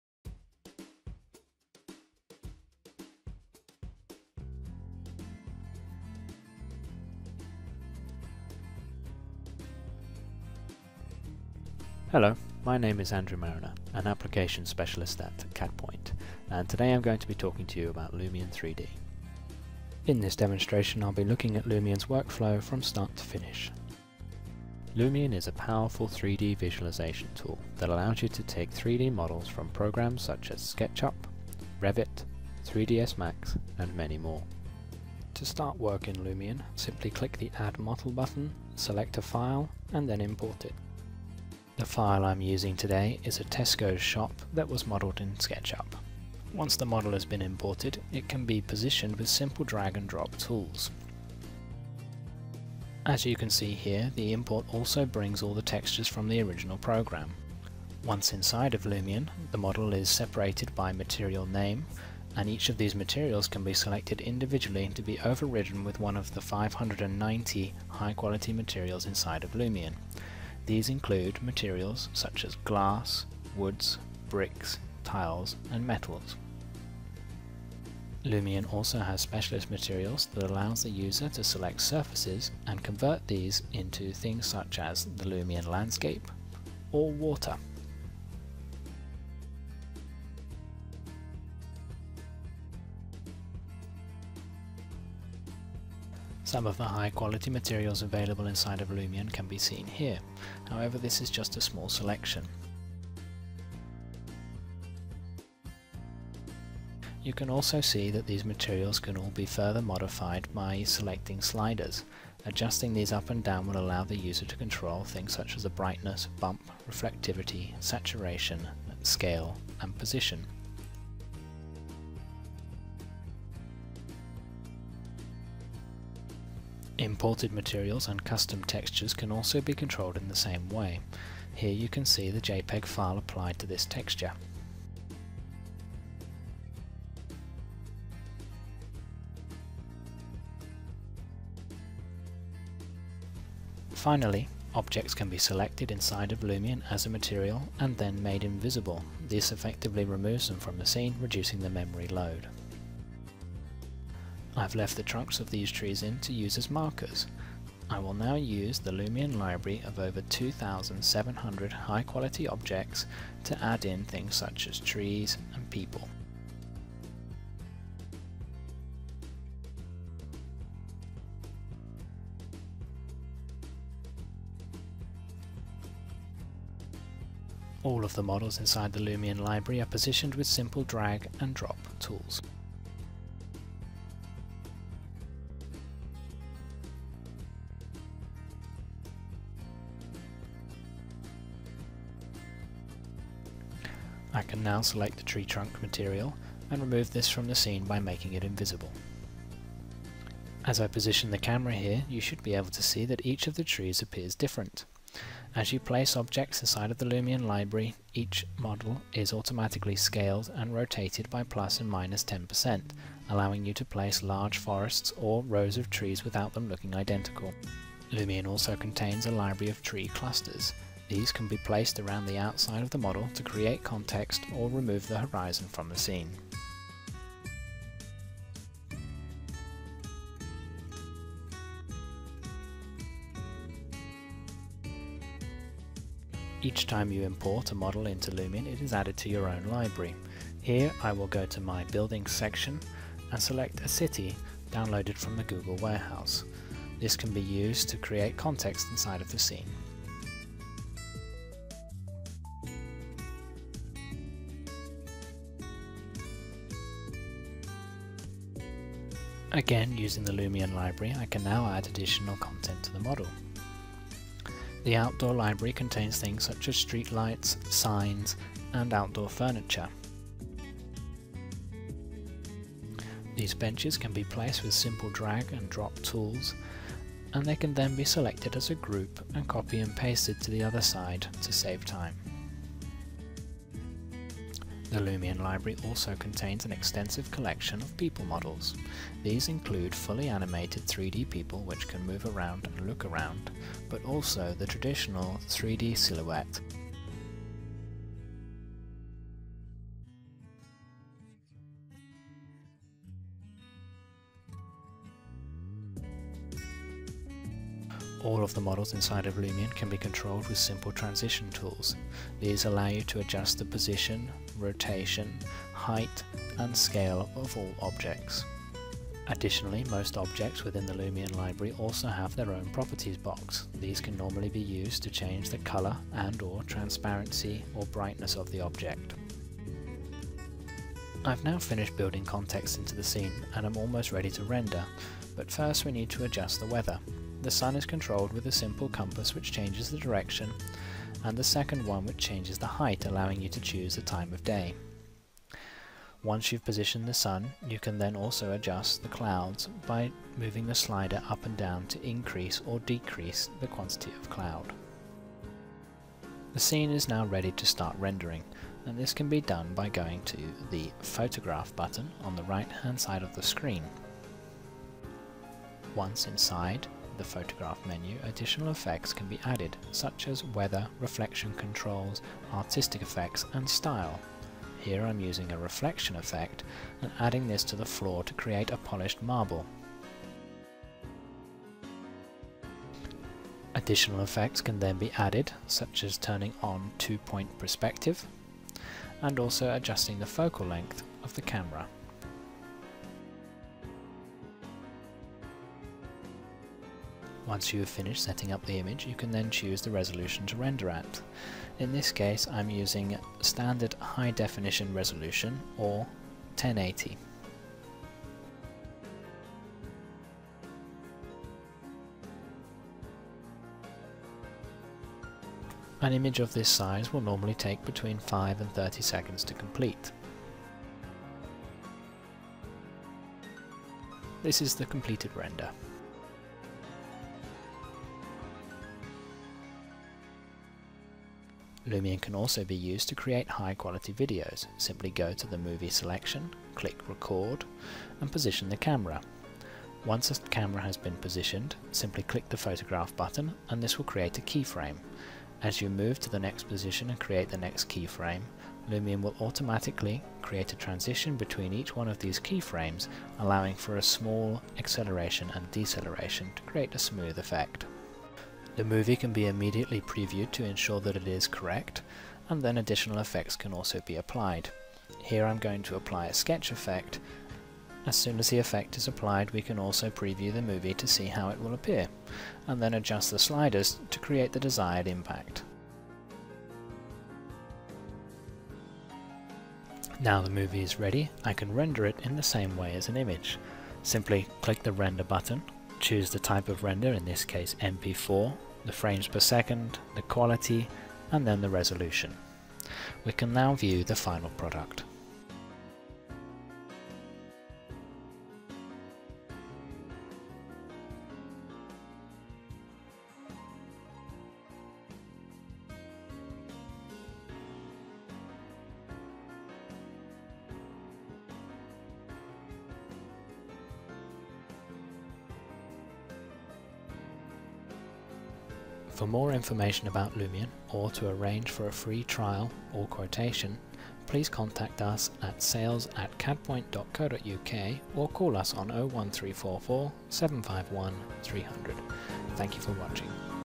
Lumion Workflow - How it Works
This short video explores the basic Lumion workflow, focusing on the start to finish process of building and populating a scene and producing a render with Lumion.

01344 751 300